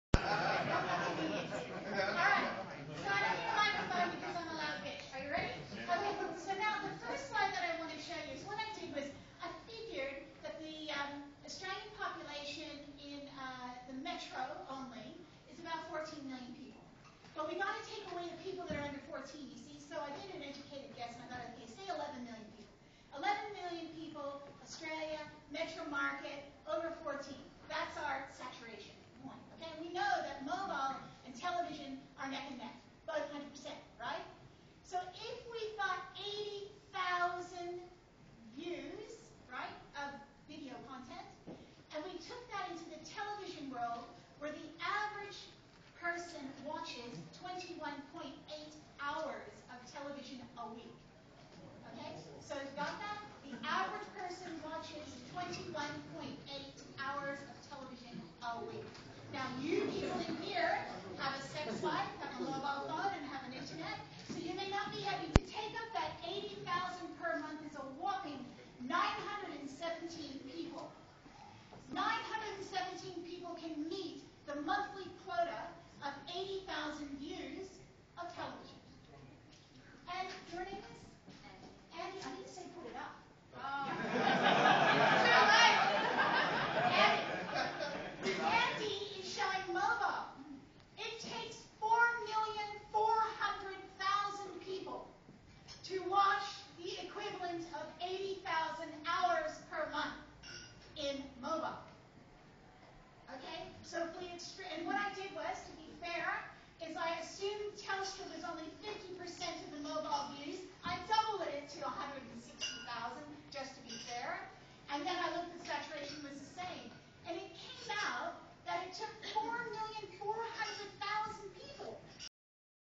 Robbee Spadafora at Mobile Monday Melbourne
Robbee Spadafora at Mobile Monday Melbourne, June 2008, with her interesting presentation style on Mobile TV